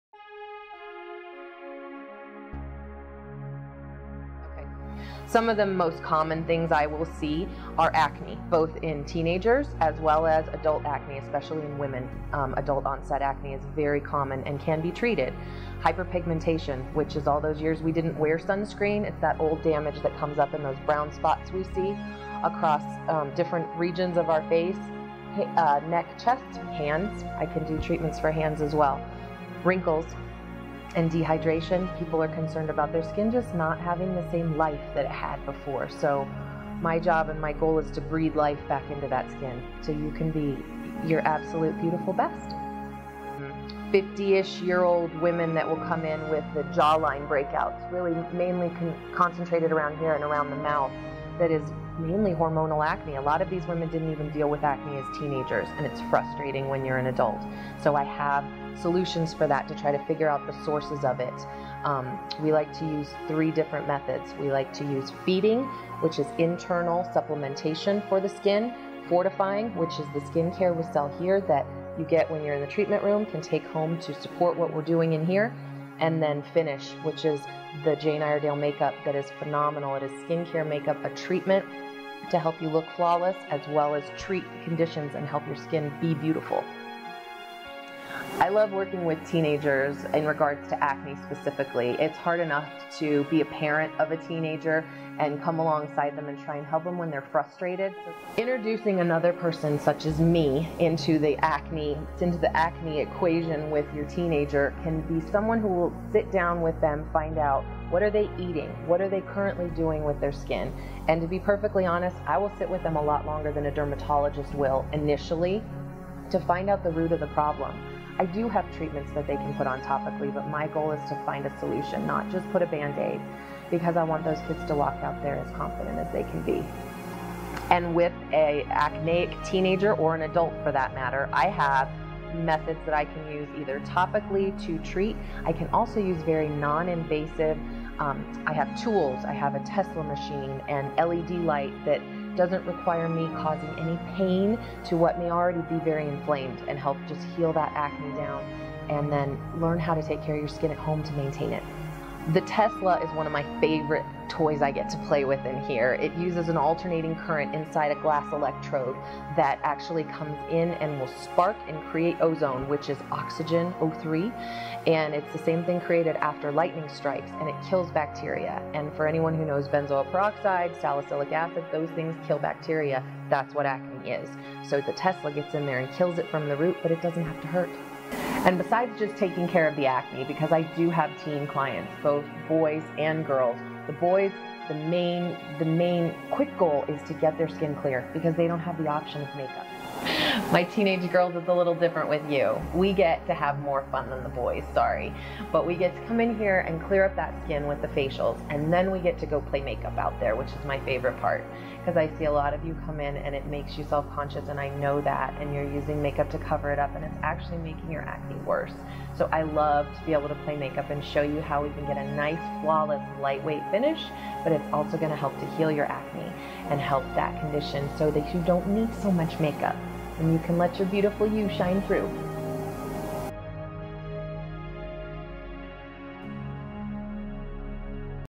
Acne
Business website video for Fierce Beauty in Colorado Springs for adult and teenage acne